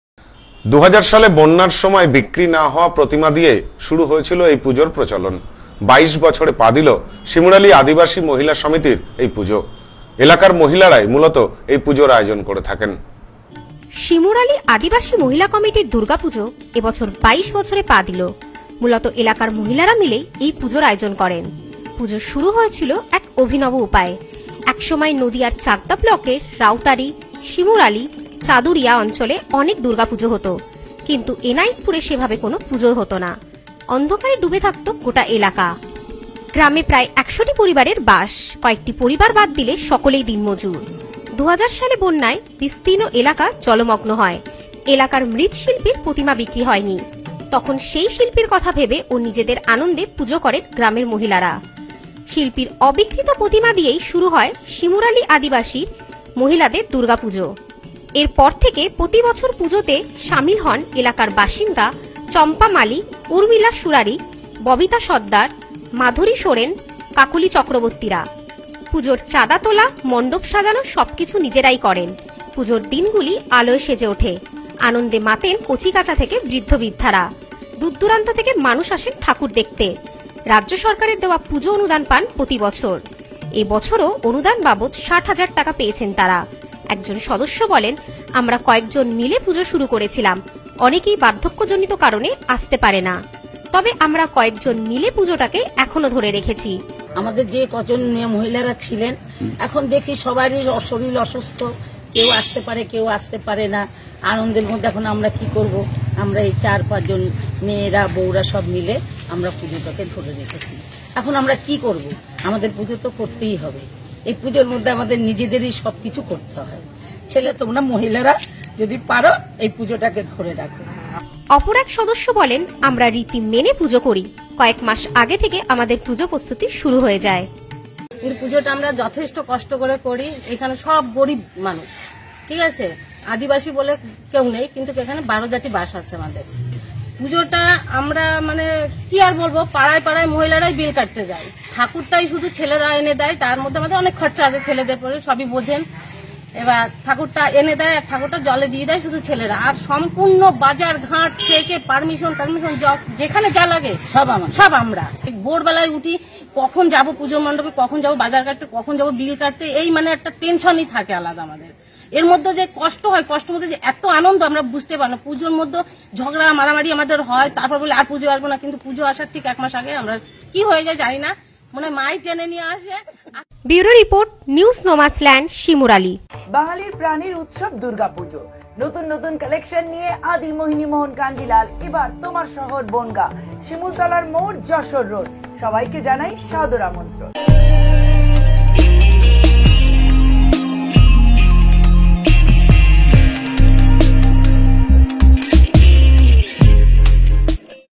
বন্যার সময় বিক্রি না হওয়া প্রতিমা দিয়ে এই পুজোর শুরু
বন্যার সময় বিক্রি না হওয়া প্রতিমা দিয়ে পুজোর শুরু ।বাইশ বছরে পা দিল শিমুরালি আদিবাসী মহিলা কমিটির  দুর্গাপুজো।